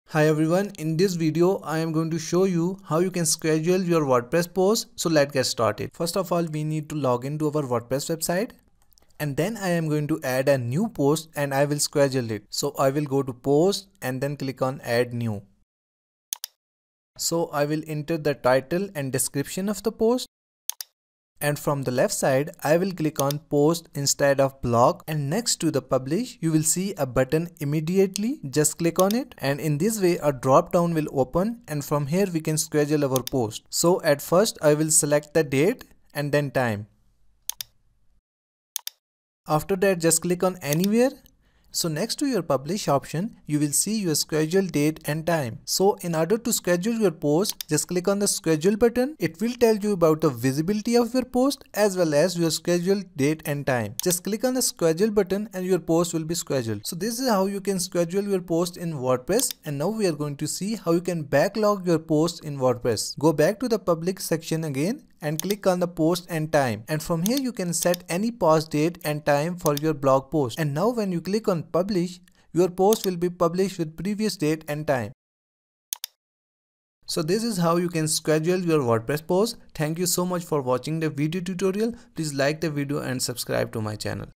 How To Schedule Post In WordPress 2025 | WordPress Post Scheduling
I will guide you on How To Schedule Post In WordPress or WordPress Post Scheduling in a very easy way. WordPress scheduled post or WordPress Post Scheduling is best for you if you want to publish posts even in the future.